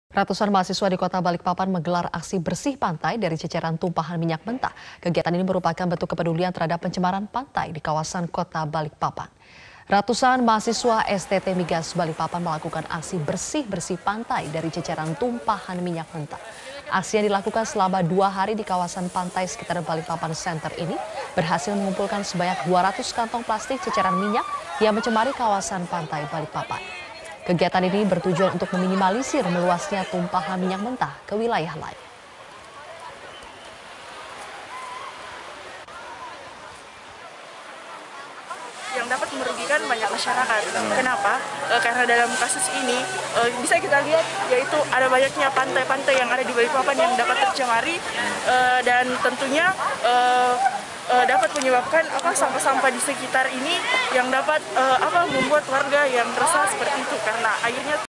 Ratusan Mahasiswa Bersihkan Tumpahan Minyak di Balikpapan
Ratusan mahasiswa di Kota Balikpapan menggelar aksi bersih pantai dari ceceran tumpahan minyak. Kegiatan ini merupakan bentuk kepedulian terhadap pencemaran pantai di kawasan Kota Balikpapan.